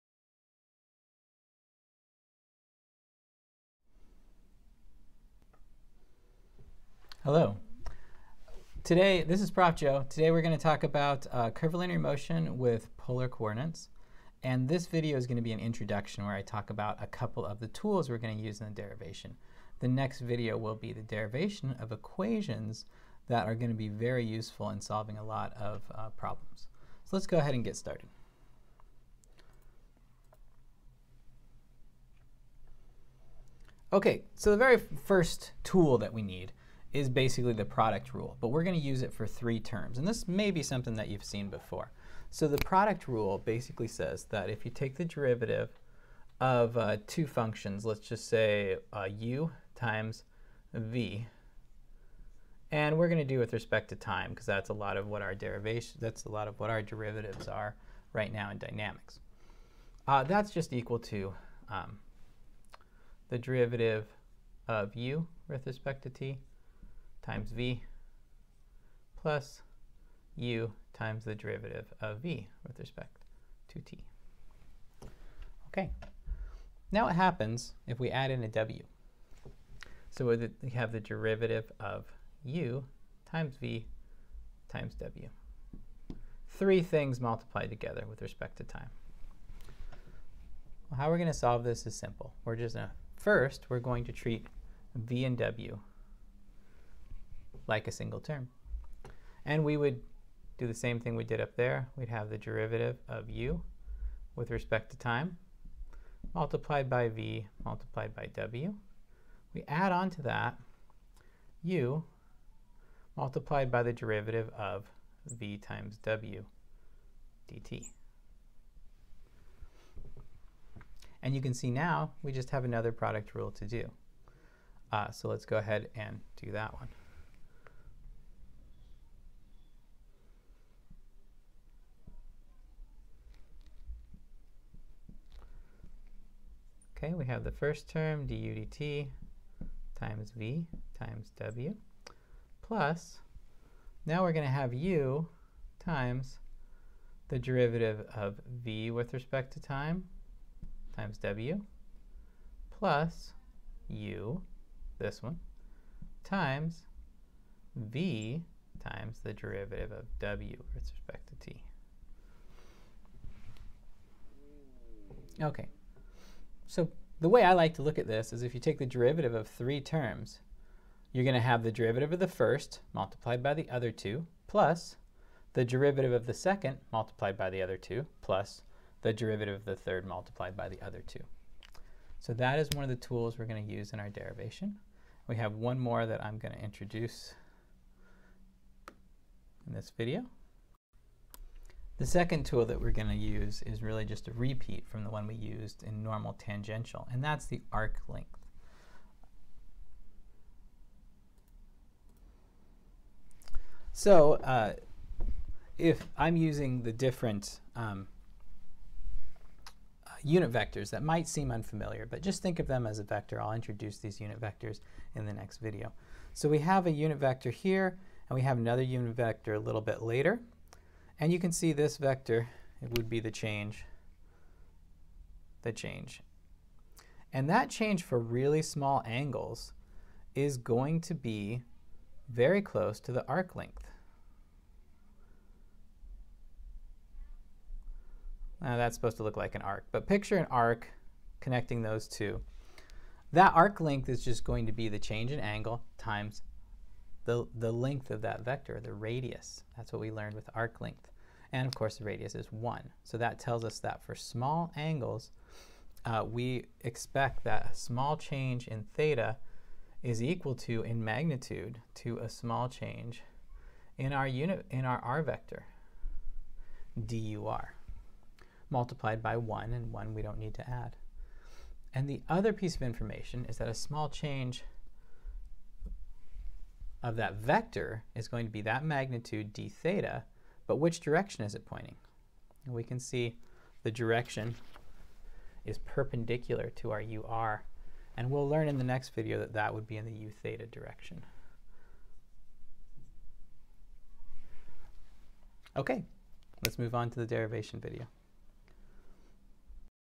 Curvilinear motion with Polar Coordinates Introduction
In this video I will review two tools that we will be using to derive the velocity and acceleration equations for Polar Coordinates. The two tools are the product rule for three terms and using arc length to find the rate of change of a unit vector.